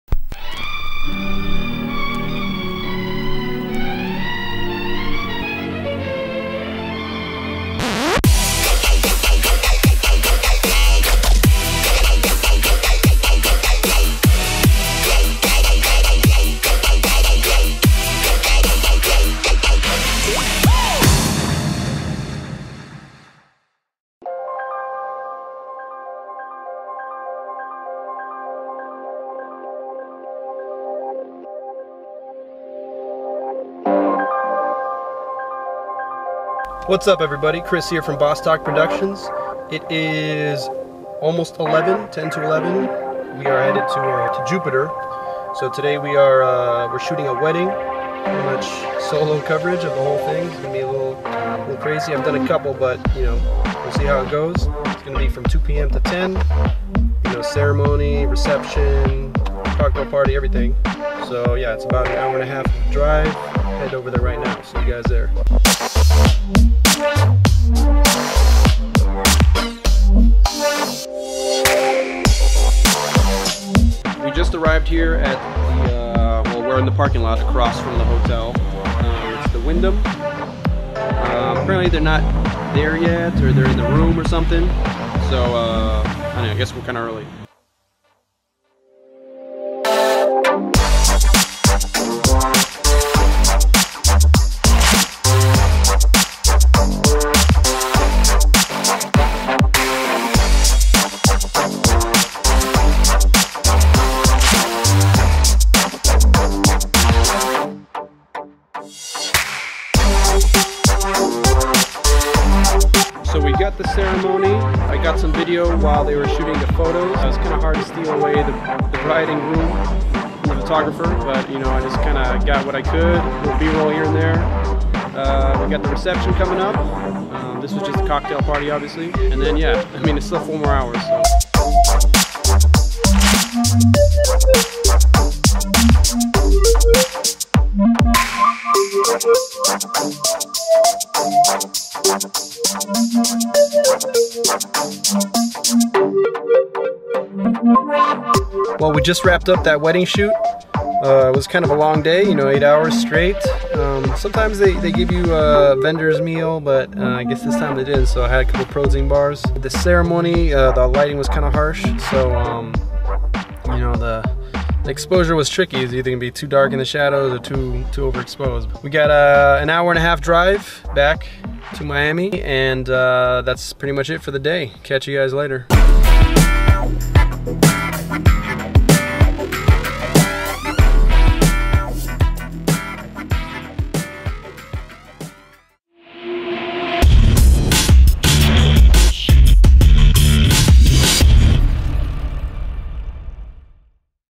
Wedding in Jupiter - Vlog 44
In episode 44 we drive up to Jupiter to shoot a wedding. Long day but we got some great footage.